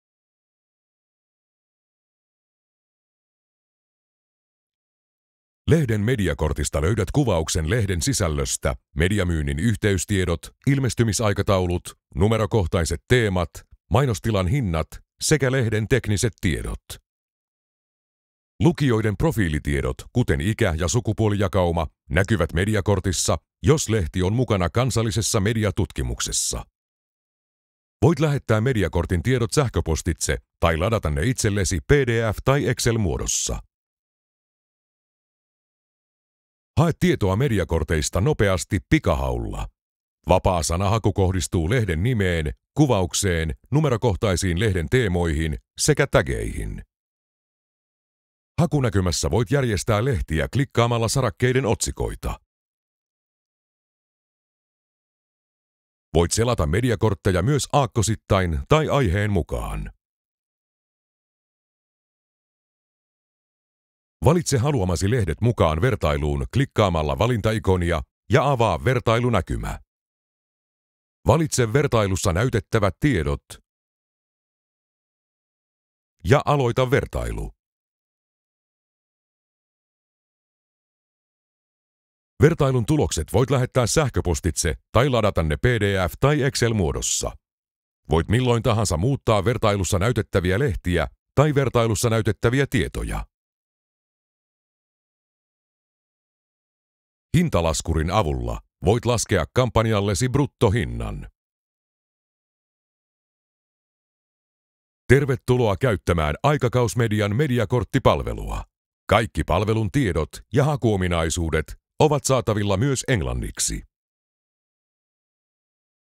Näin käytät Mediakorttipalvelua
Aikakausmedian Mediakortit.fi-sivusto auttaa sinua löytämään tarvitsemasi tiedot entistä helpommin. Voit hakea noin 250 aikakauslehden tietoja useilla eri hakukriteereillä: aakkosten, lehtiluokkien, asiasanojen ja vaikkapa teemojen perusteella. Pystyt myös vertailemaan valitsemiesi lehtien tietoja keskenään ja laskemaan kampanjallesi kontaktihinnan. Saat vertailuun lisäksi KMT:ssa olevien lehtien profiilitiedot. Haetut tiedot siirrät vaivattomasti Excel- tai PDF-tiedostoihin tai sähköpostiin.

00:04 Mitä tietoa mediakortista löytyy?
00:27 Mediakortin tietojen exporttaus
00:36 Näin haet tietoa mediakorteista: vapaahaku, aakkosellinen ja aiheittain selaaminen
01:04 Lehtien tietojen vertailu
01:21 Vertailun tulosten exporttaus
01:35 Kampanjan bruttohinnan laskeminen